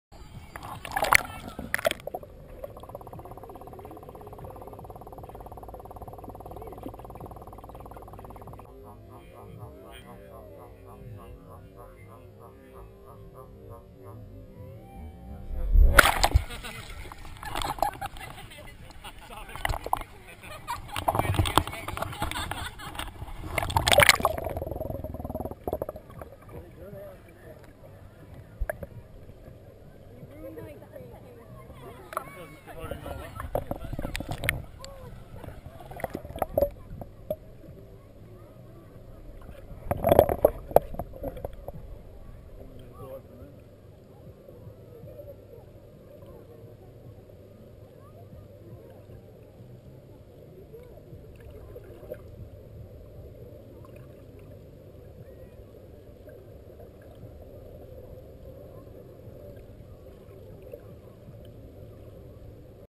Seaworld Rays
The rays at SeaWorld , Gold Coast Australia